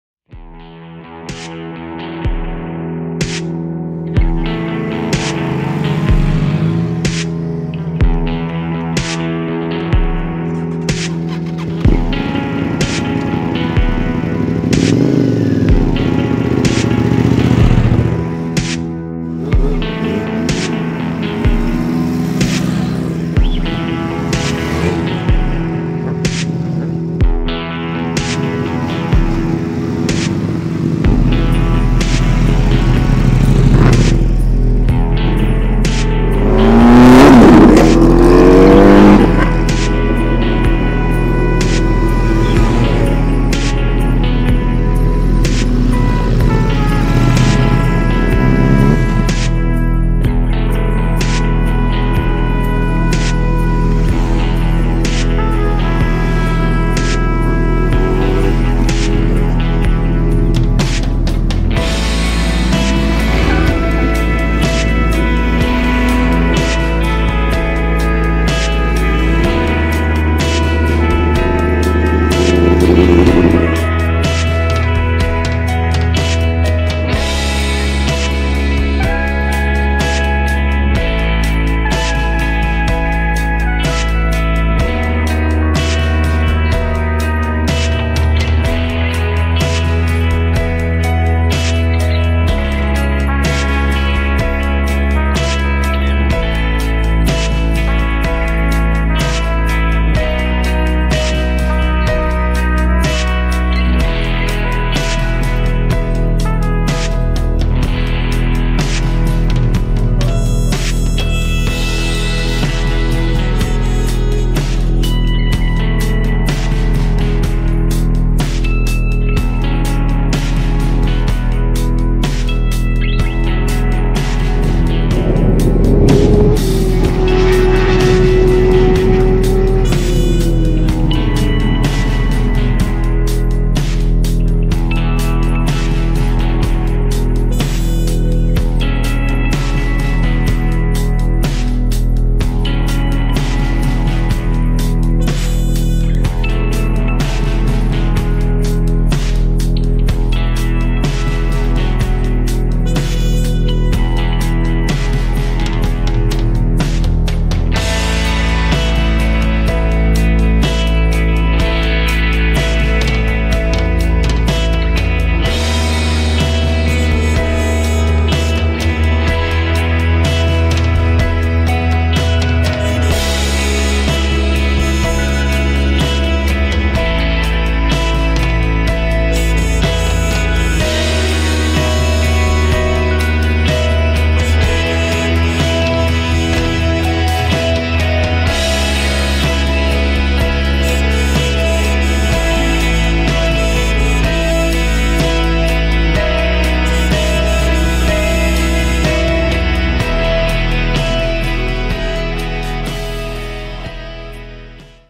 Wauhtipäivä 12.5.2018
Nopeusmittaus moottoripyörille.Nopeus mitataan tutkalla 1/4 mile.
Rentoa meininkiä ,aurinkoa,hienoja pyöriä.